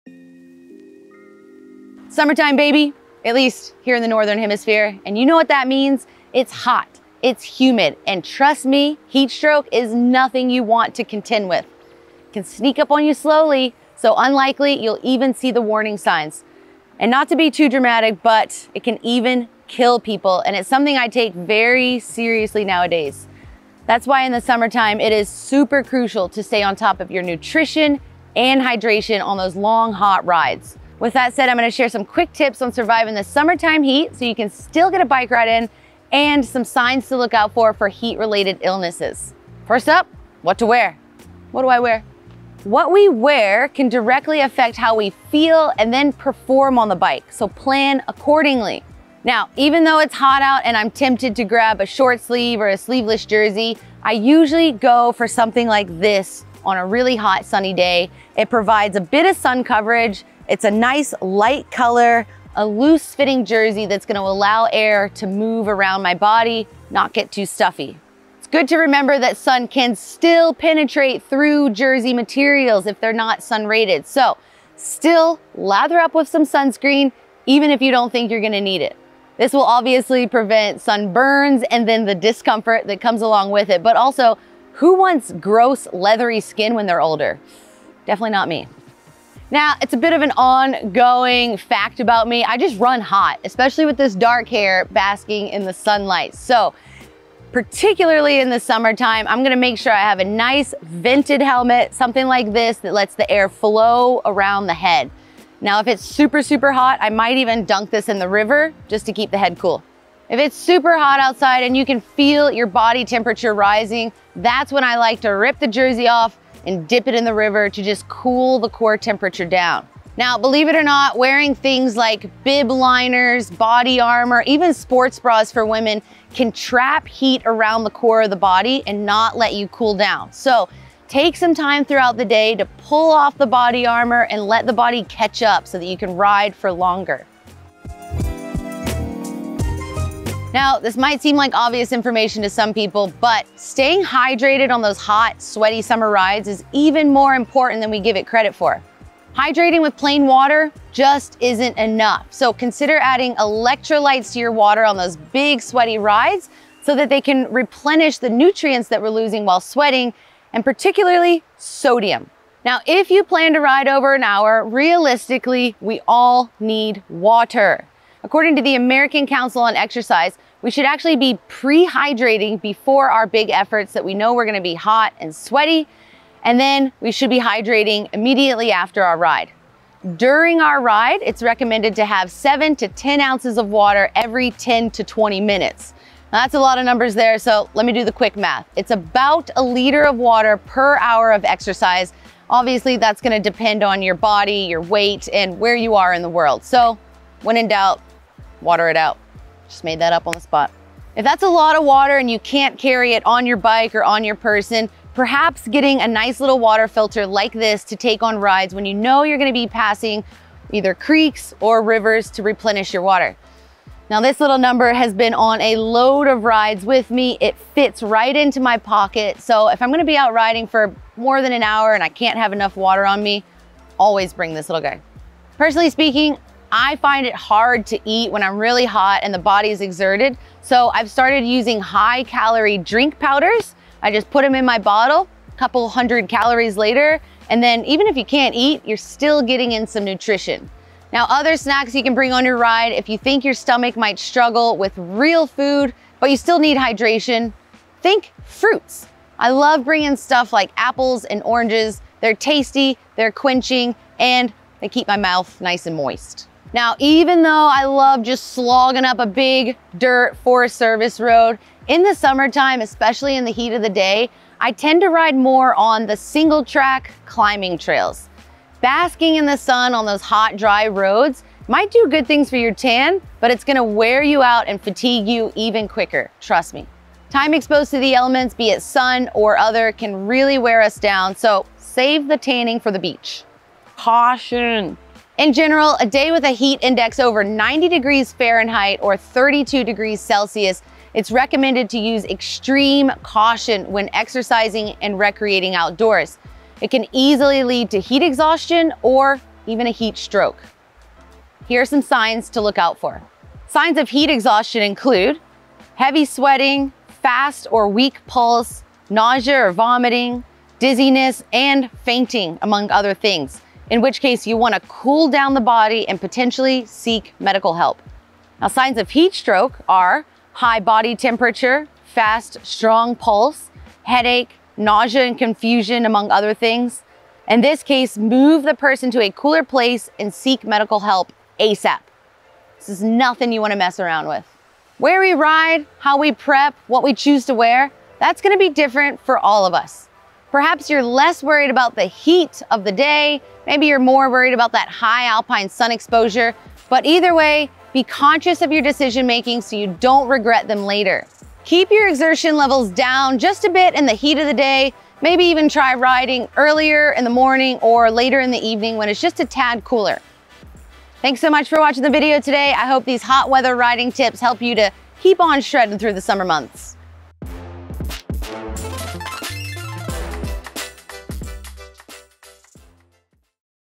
10 Tips for Riding in the Heat: Stay Cool and Bike All Summer Long
Christina goes through some easy to incorporate methods of staying cool, and riding safer and for longer on those hot summer days.

Get unlimited access to all the best mountain biking trails by clicking the link above.

0:00 - Intro
0:42 - What to Wear
1:31 - Manage the Heat
2:20 - Hydration
3:45 - Trail Food
4:45 - Caution
5:03 - Signs of Heat Exhaustion
5:18 - Signs of Heat Stroke
5:34 - Summary